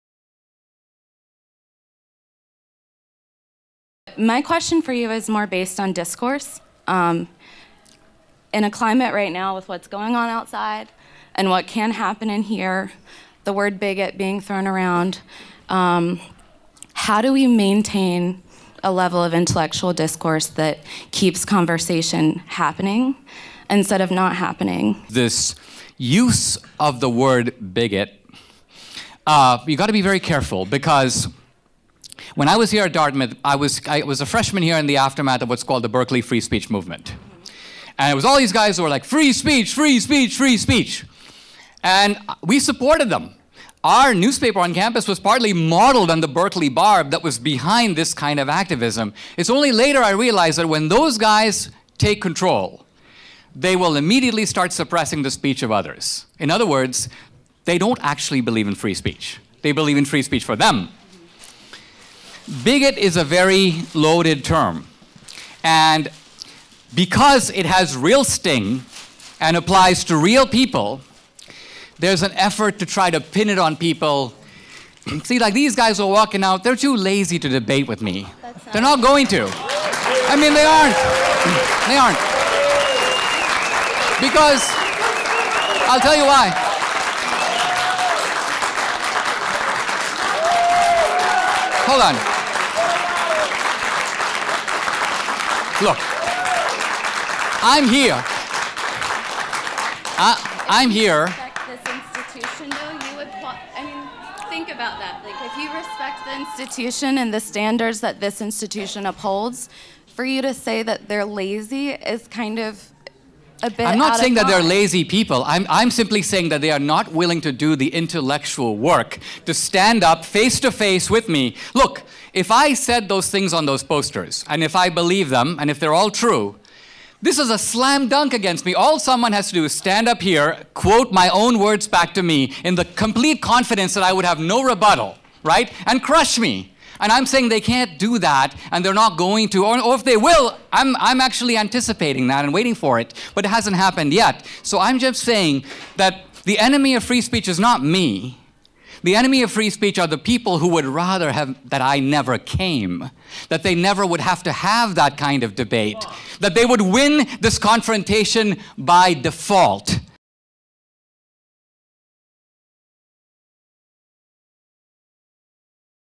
SHUT. DOWN. Dinesh D'Souza 100% owns snowflakes trying to stage a "walk out"
SHOTS FIRED: "I am simply saying they are not willing to do the intellectual work to stand face-to-face with me."

Dinesh D'Souza leaves nothing unsaid at Dartmouth!